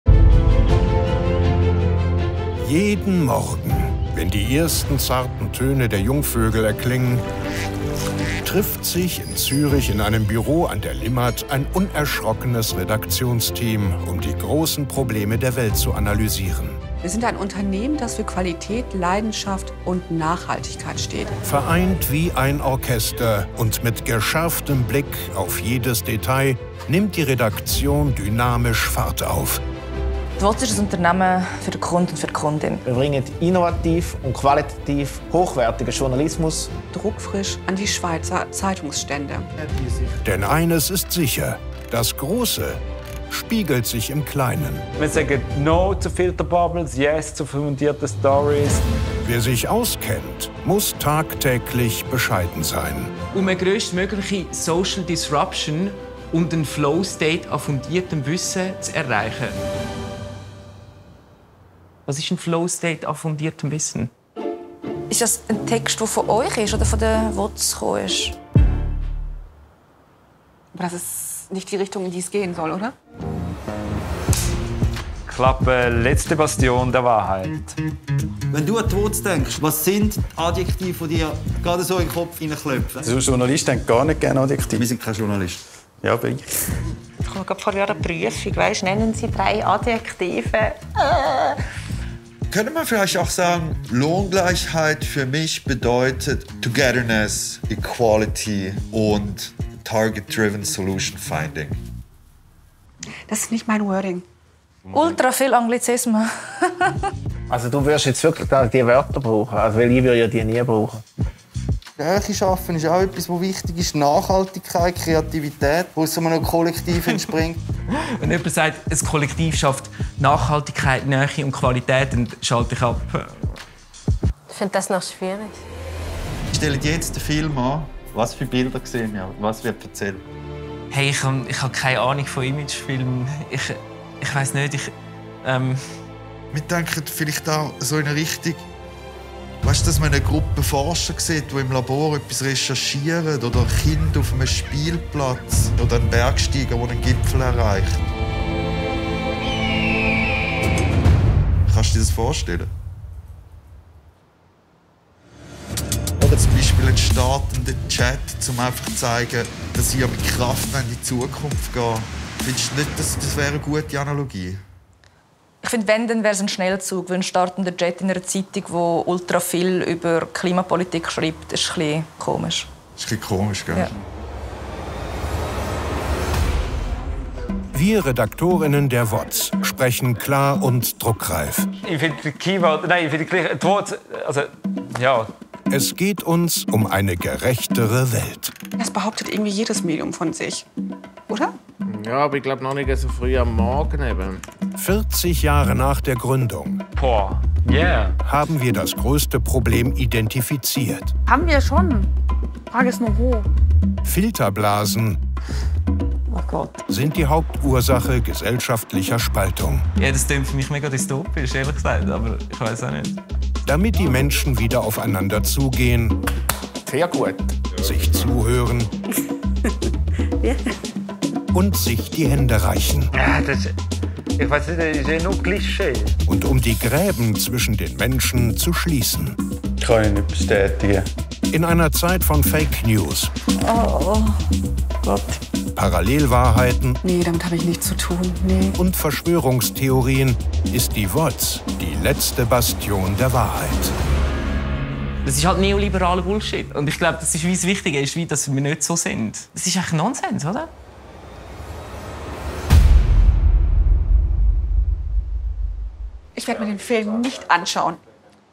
WOZ: Der Imagefilm
Mit solch «neoliberalem Bullshit» hatten die WOZ-Redaktor:innen und Verlagsmitarbeiter:innen also wirklich nicht gerechnet: Schöne Bilder aus dem Grossraumbüro an der Limmat, ein paar Fragen zum Redaktionsalltag, das war zu erwarten. Aber dann kam alles ganz anders, die Medienprofis des WOZ-Kollektivs gerieten für einmal ins Schwitzen. Bei einem Kraftakt von Dreh ist nichts weniger herausgekommen als die einzig wahre Wahrheit über das grösste Medienkollektiv der Schweiz.

Idee & Konzept: Jovica, Aron Marty (docyard GmbH)
Regie: Jovica
Kamera: Aron Marty
Produktion: Andi Bänziger (docyard GmbH)
Provocateur: Mario Hipleh
Verantwortlich bei der WOZ: Claudia Gillardon, Uğur Gültekin, Camille Roseau